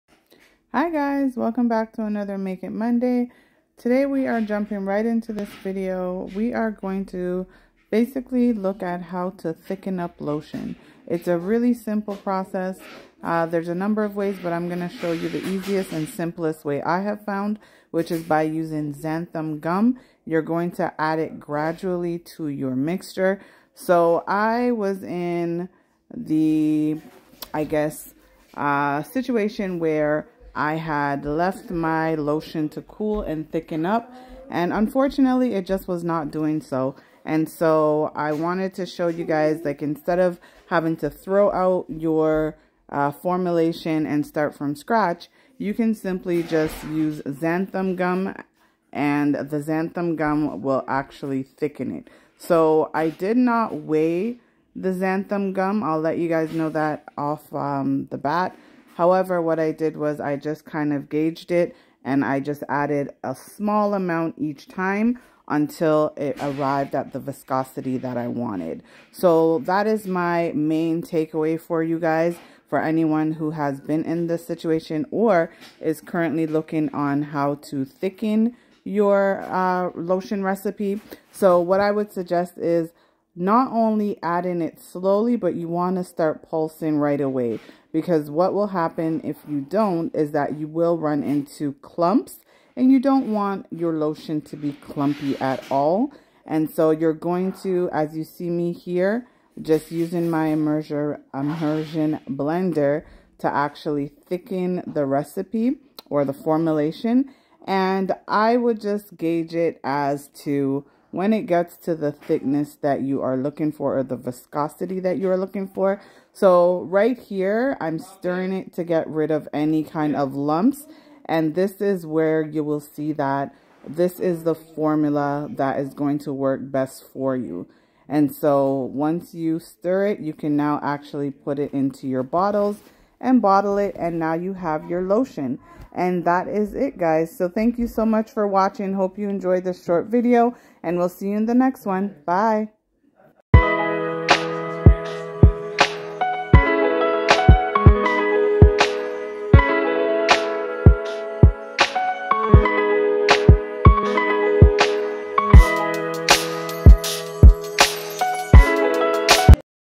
How to Thicken Up Lotion
Welcome friends! Today we are going to look at a quick fix for when your lotion formulation is too thin or runny.